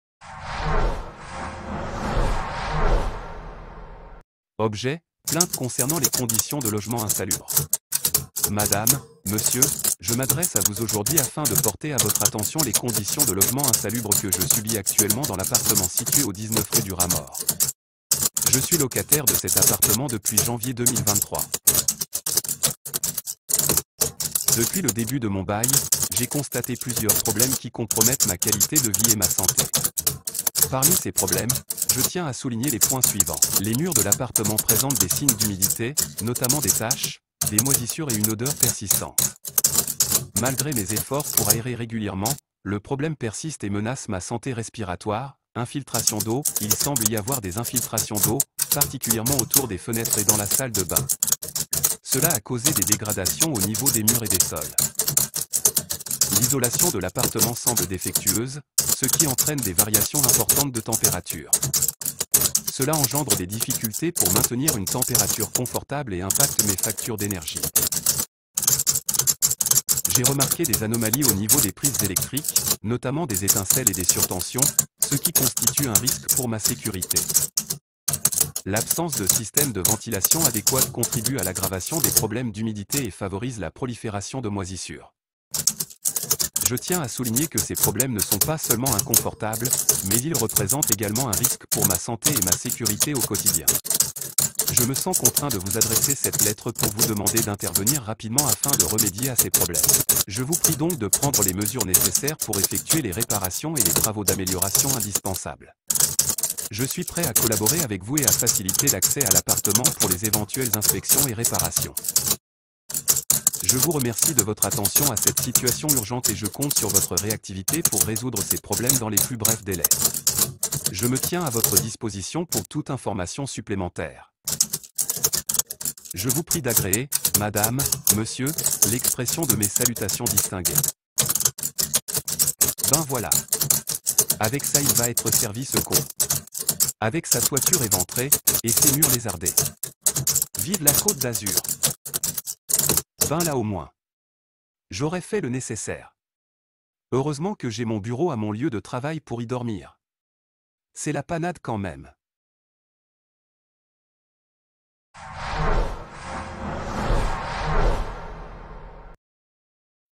Reallusion 7.9 Social et la Vie Insalubrité : Lettre de Plainte
Iclone Creator 3  HeadShot 1
Composé une lettre lu oralement pour les services de logement insalubre.